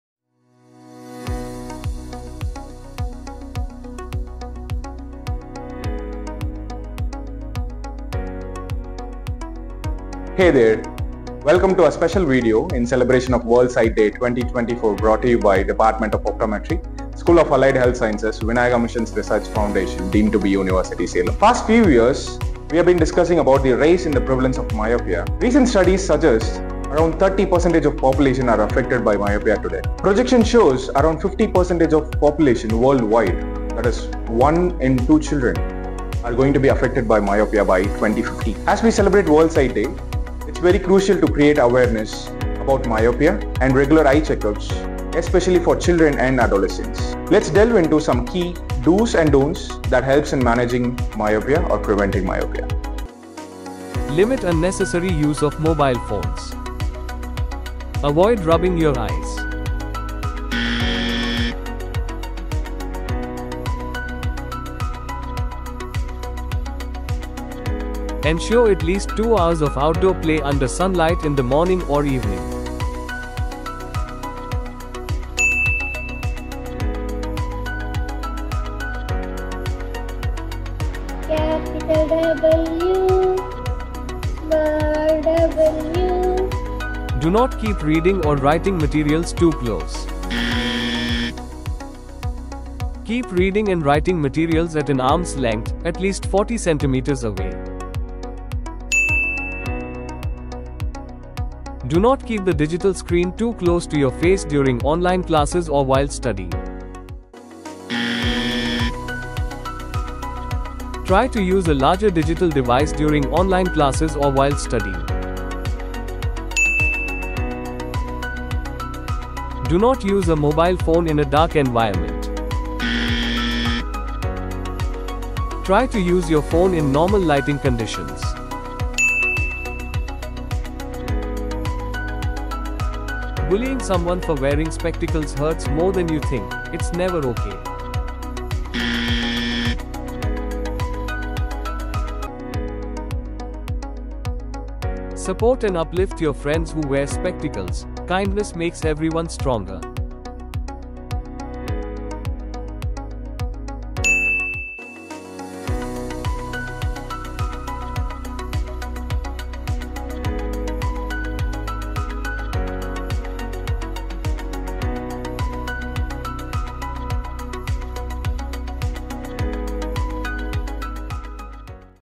World Sight Day 2024 | Myopia Awareness – Part 2 | Do's and Don'ts
World Sight Day 2024 | Myopia Awareness – Part 2 | Children’s Do's and Don'ts

As part of our World Sight Day 2024 celebration, the Department of Optometry at the School of Allied Health Sciences, Vinayaka Mission's Research Foundation, is proud to present Part 2 of our Myopia Awareness campaign.

In this video, we focus on essential do’s and don’ts to help children prevent, manage, and control myopia. From lifestyle tips to healthy screen habits, this guide offers practical advice for parents and children alike to protect their vision.

Watch to learn: 🔹 Simple steps to prevent myopia
🔹 Effective ways to manage and control existing myopia
🔹 Key lifestyle habits that support eye health

Encourage regular eye check-ups and make informed choices for a myopia-free future!